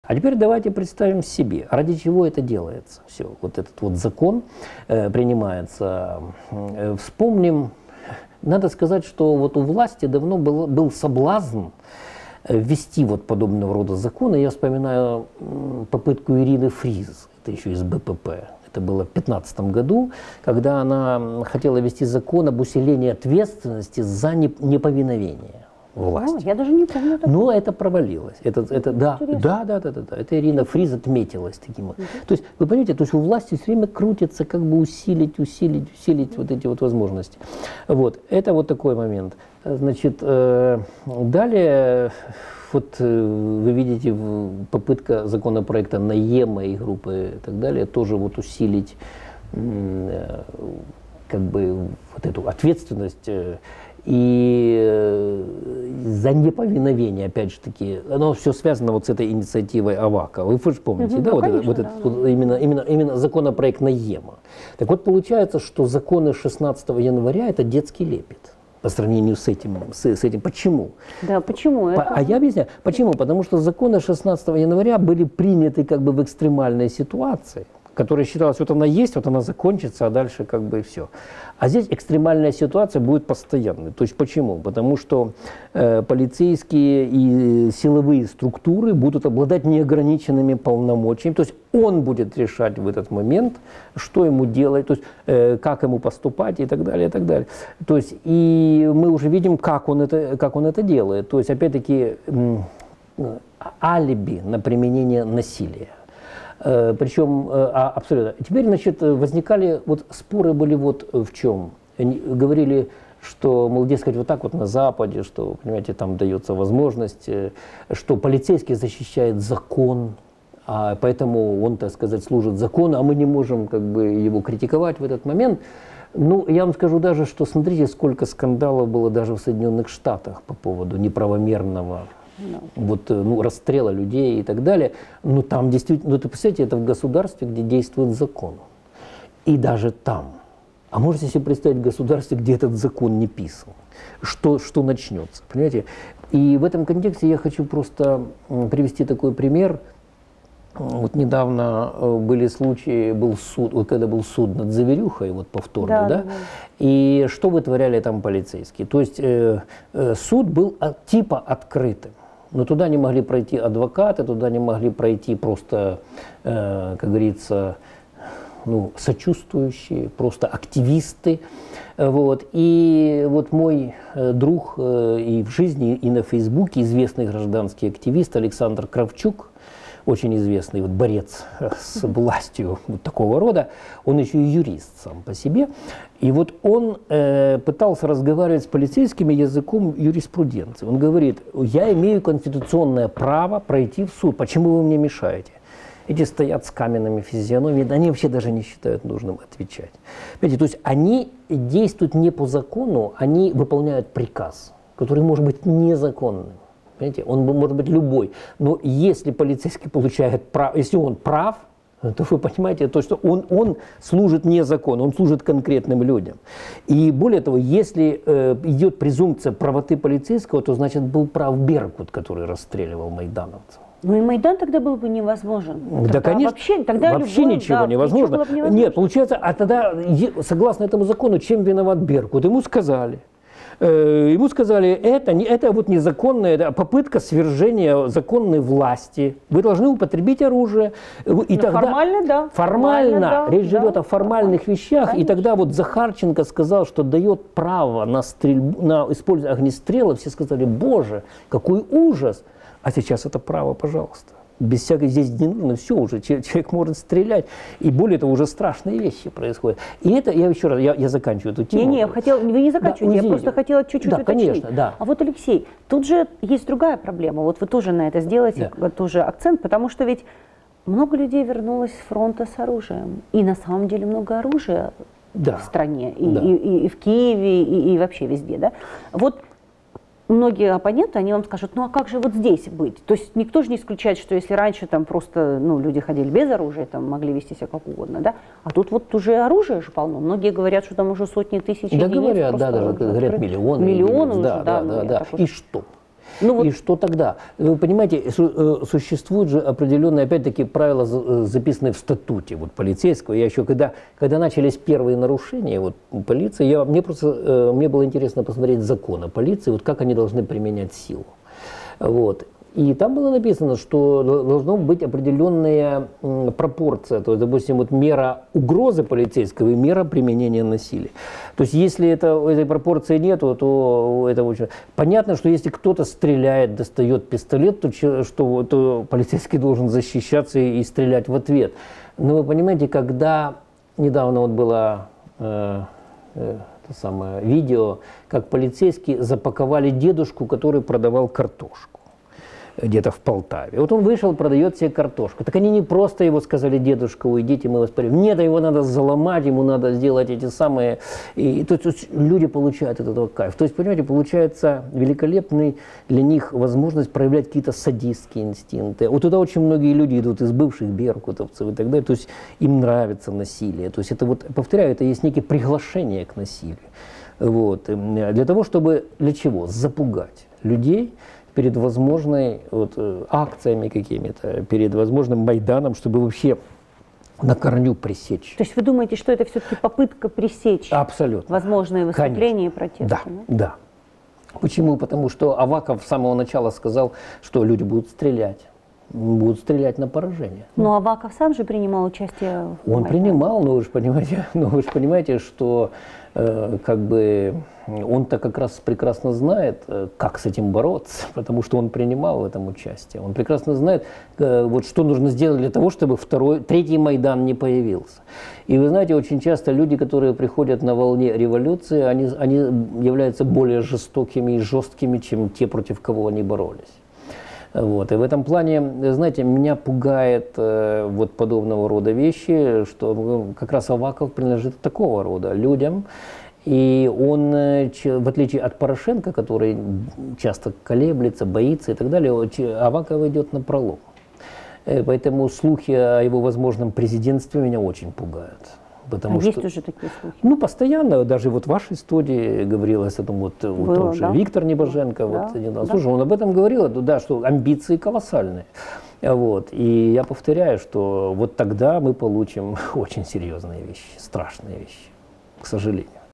Принятие закона о презумпции правоты полицейского – попытка пресечь возможные протесты и выступления
Философ и политолог Алексей Шевченко о том, что может значить принятие закона о расширении полномочий полицейских.

UKRLIFE.TV - это события политики, экономики и культуры в комментариях экспертов, аналитиков, общественных активистов, журналистов, действующих лиц украинской жизни.